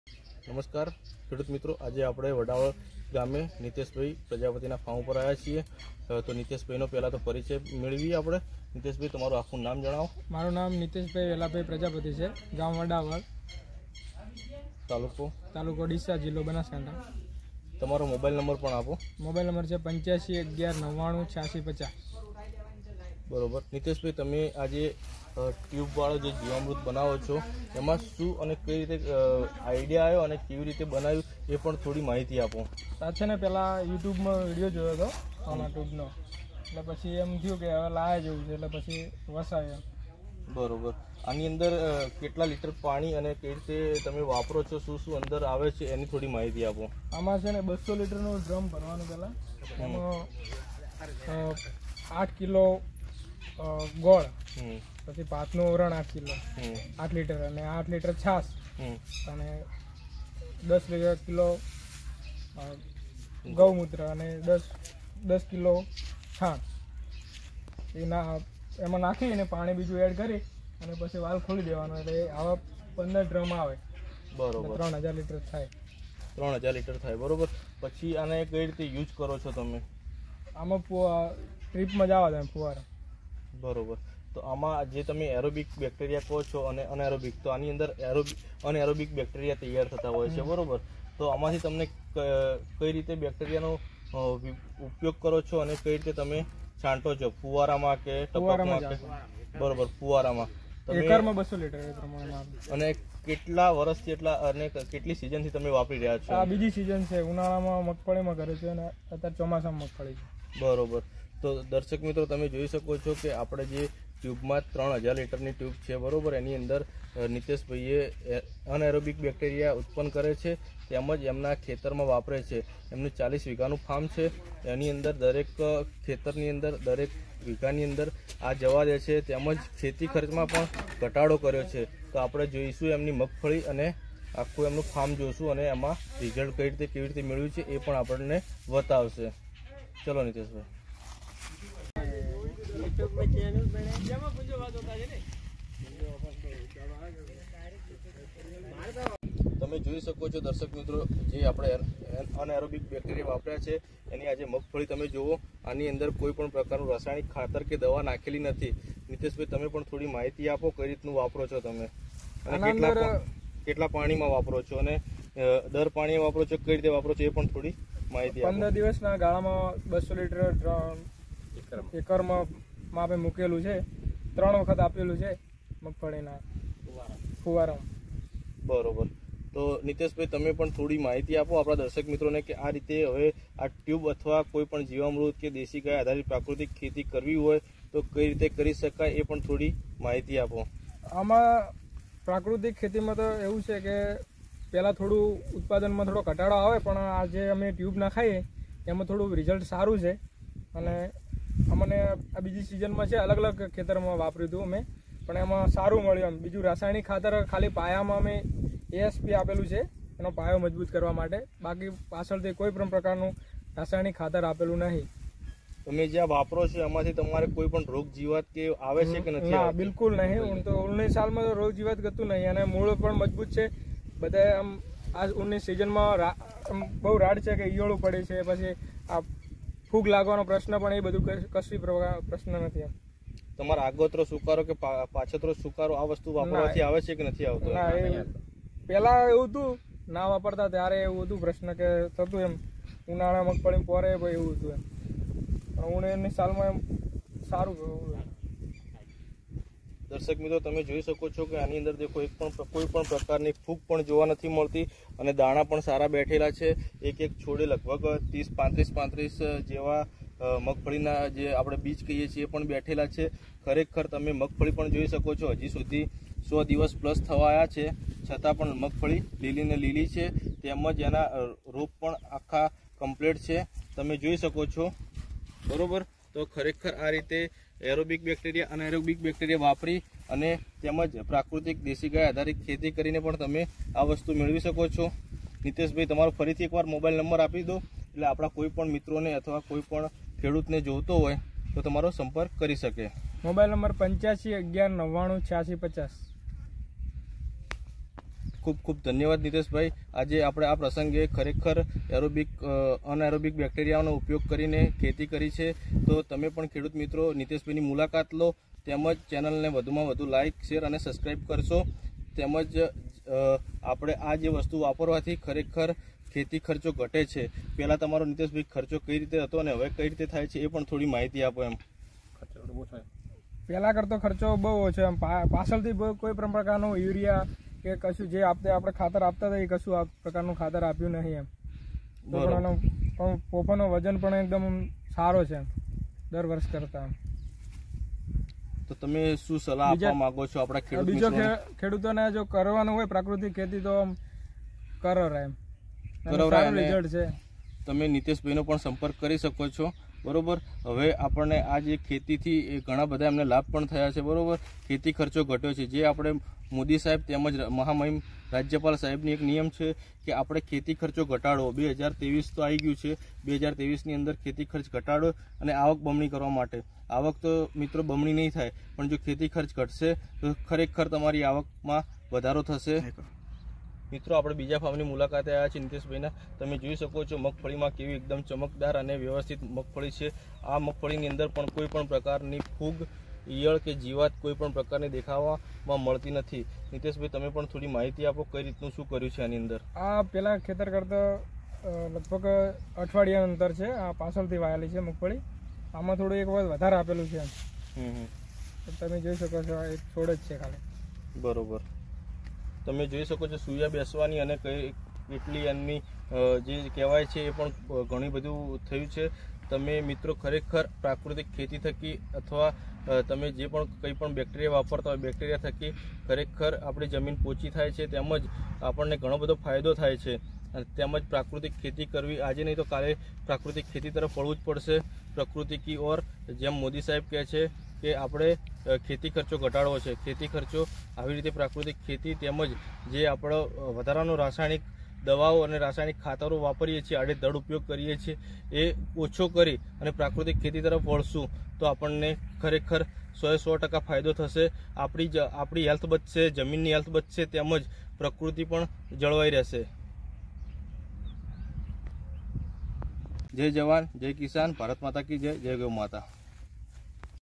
અનએરોબિક બેક્ટેરિયા દ્રારા પ્રાકૃતિક ખેતી
નિતેશભાઈ પ્રજાપતિ
ગામ - વડાવલ
તા- ડીસા
જી - બનાસકાંઠા